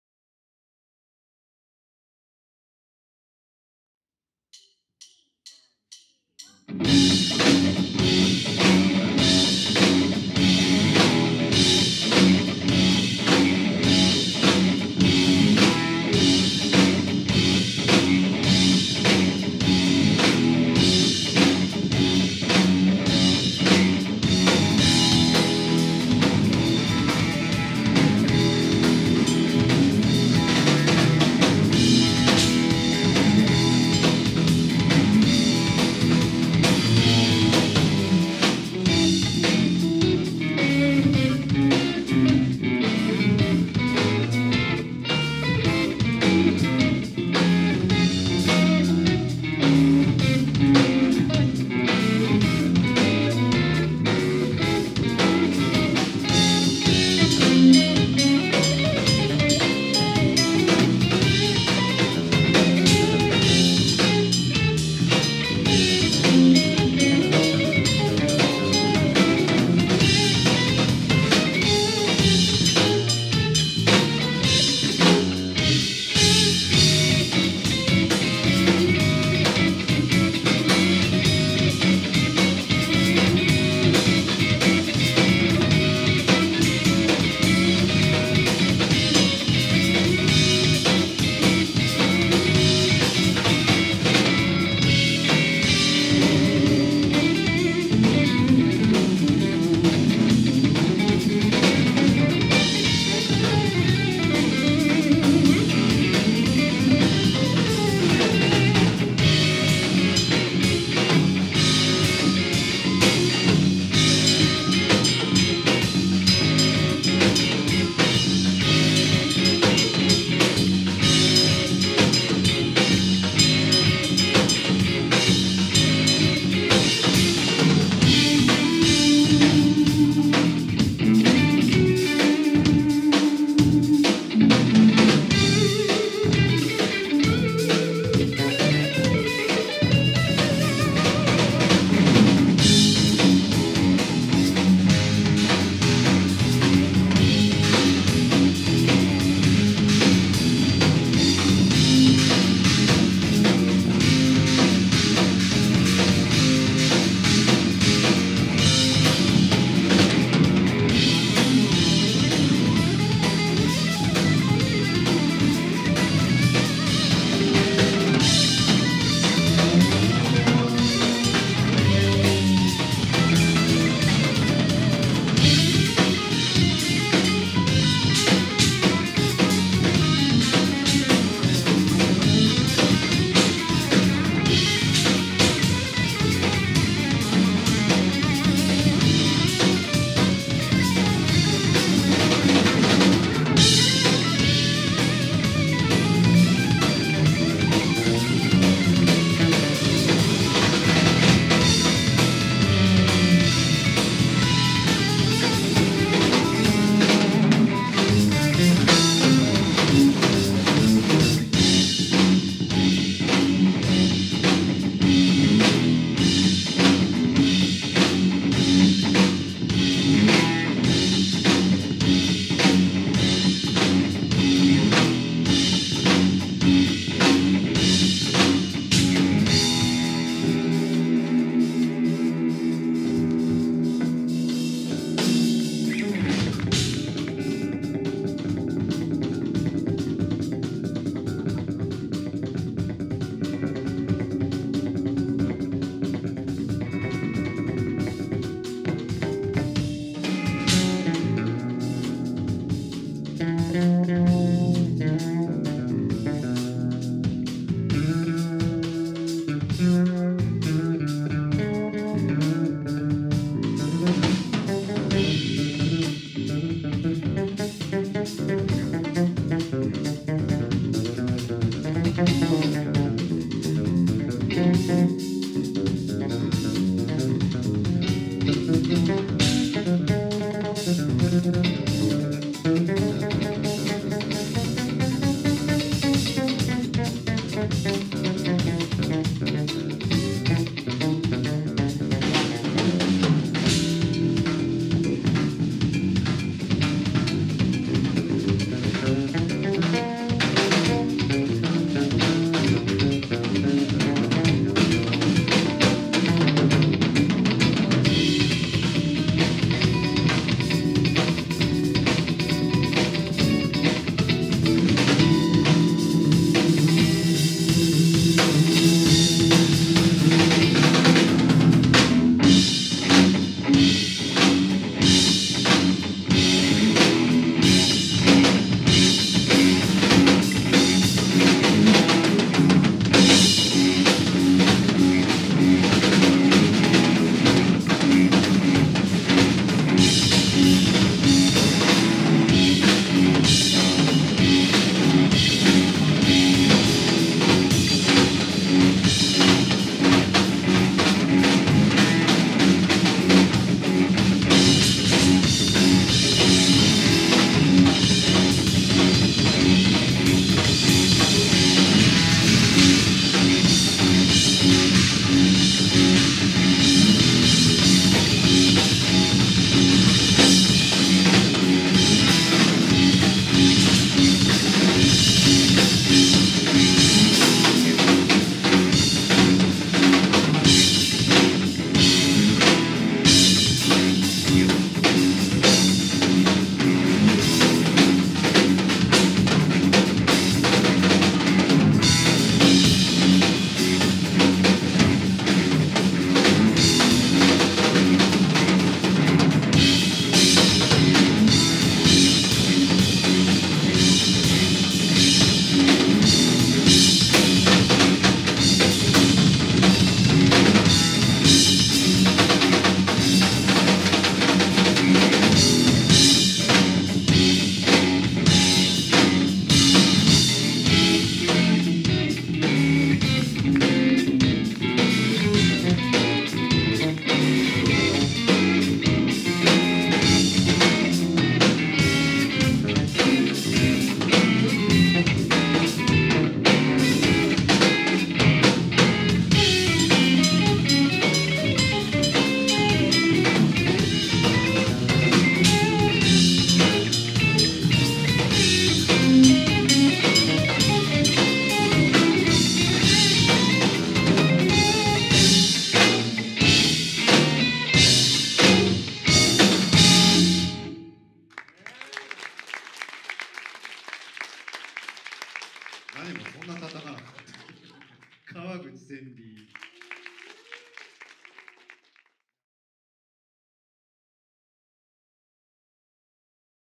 LCM:うらがえる@金沢文化ホール220821
2022年8月21日＠金沢文化ホール
セミナー会場で行われた「うらがえる」スペシャル・ライブからLCM演奏の模様です。適当な定点カメラで画角はすみませんm(_ _)m
矢堀孝一(g) 織原良次(fretless bass) 川口千里(d)

Koichi Yabori
Ryoji Orihara
Senri Kawaguchi